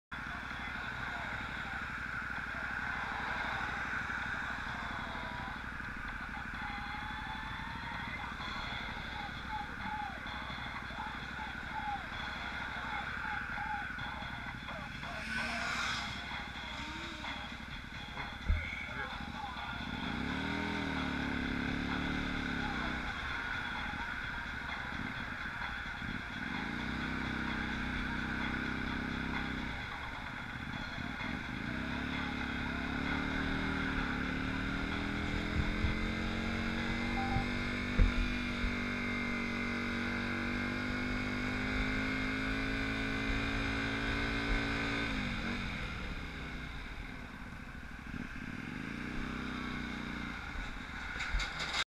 Mottram incident 12/03/2017
Incident between Motorbike and Car at top of Mottram Moor lights.